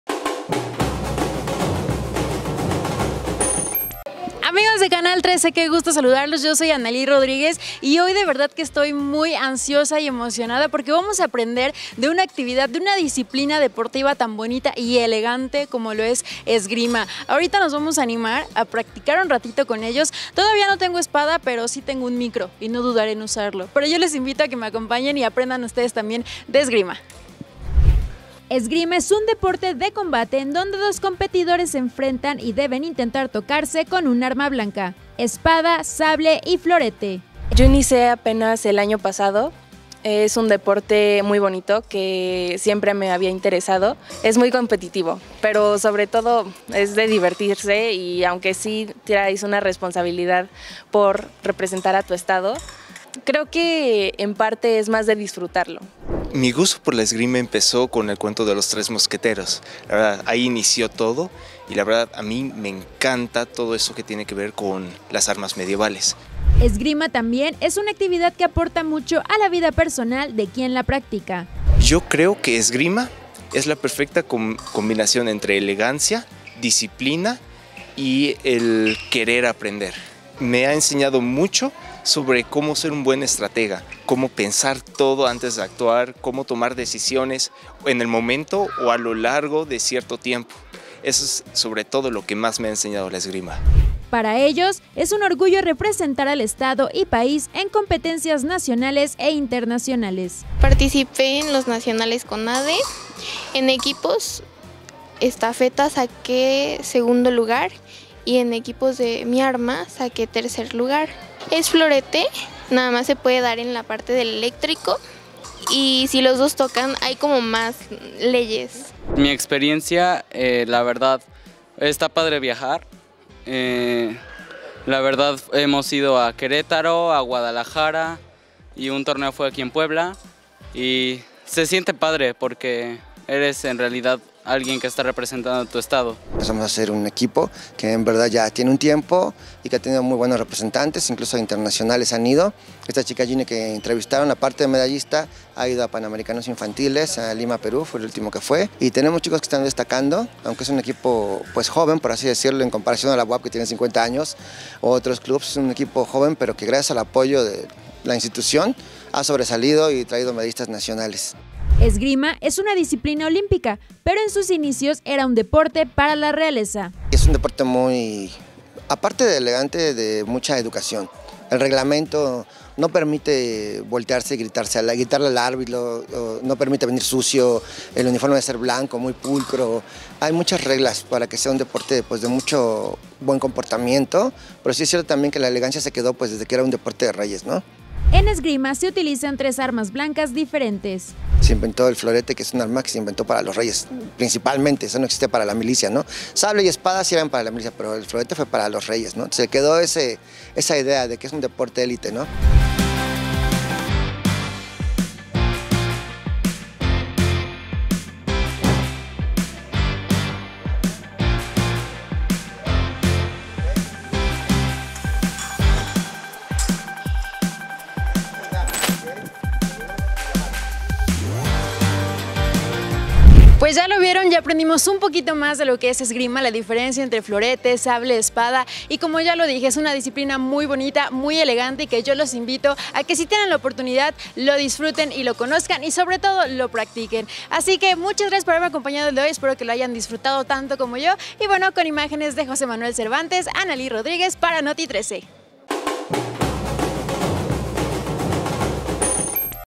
🔴 Cautiva la esgrima a poblanos.
🔴 La esgrima es un deporte que ha cautivado a muchos poblanos, que han logrado sobresalir a nivel nacional con esta actividad, y que ahora, se sienten orgullosos de pasar el mensaje, del porqué, debe practicarse y disfrutarse con pasión.
Analy Rodríguez @Canal13Puebla